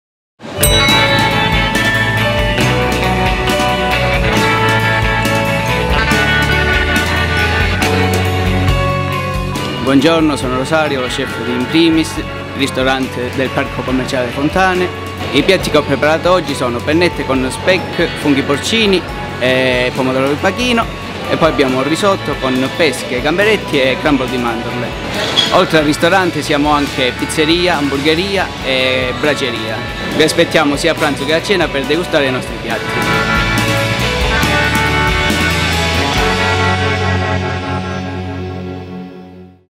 Le Fontane Food Time IN Primis
🕜 Oggi tutti a pranzo da In primis 🔝
Ecco il menù proposto dal nostro chef 👨‍🍳
- pennette con speck, funghi porcini e pomodoro pachino
- risotto con pesche gamberetti e gambo di mandorle
Che fai non vieni? Ti aspettiamo sia a pranzo che a cena 💛